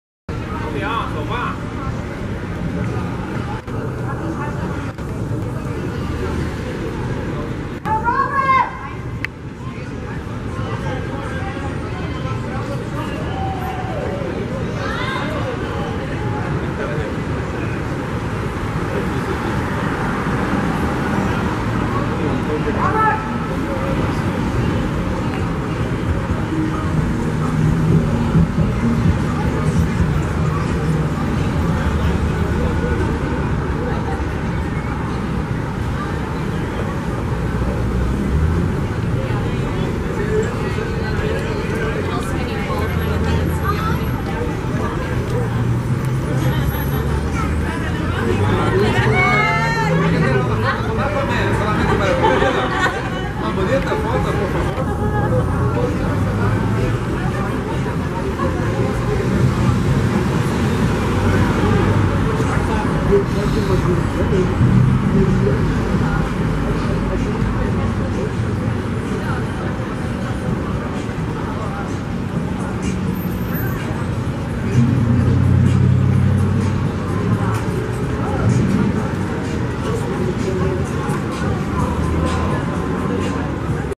Robert Downey Jr. | SR. Premiere
Robert Downey Jr (RDJ) seen on the red carpet for the film SR at AFI Fest. SR is a film about the life and career of Robert Downey Sr.